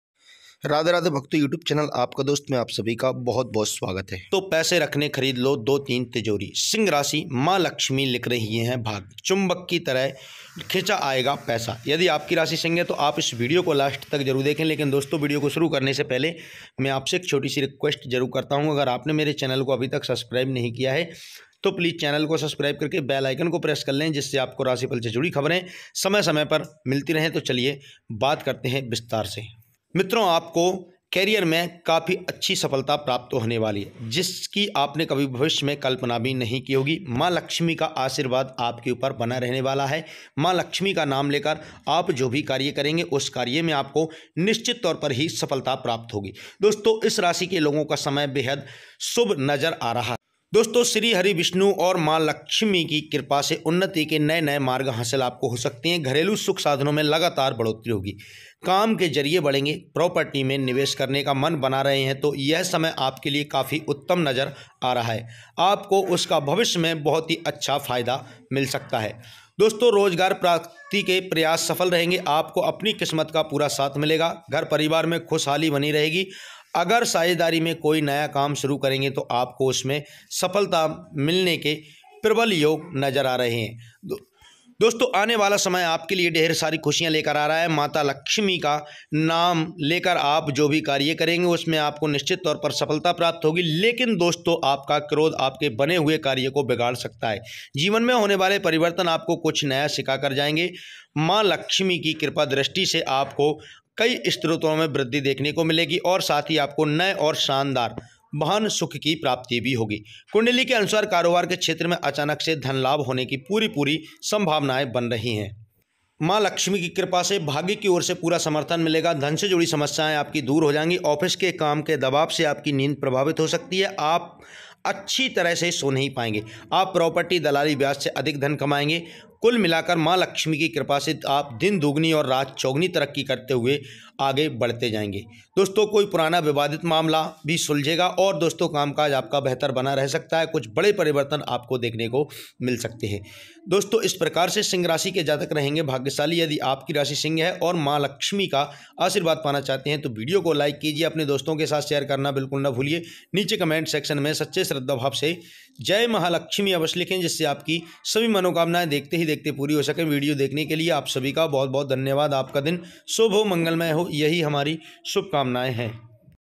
पैसे रखने खरीद लो दो-तीन तिजोरी सिंह राशि। मां लक्ष्मी लिख रही हैं भाग्य चुंबक की तरह खींचा आएगा पैस
To keep money, buy two-three vault Leo zodiac.  Maa Lakshmi is writing that luck will attract money like a magnet. ApkaDost.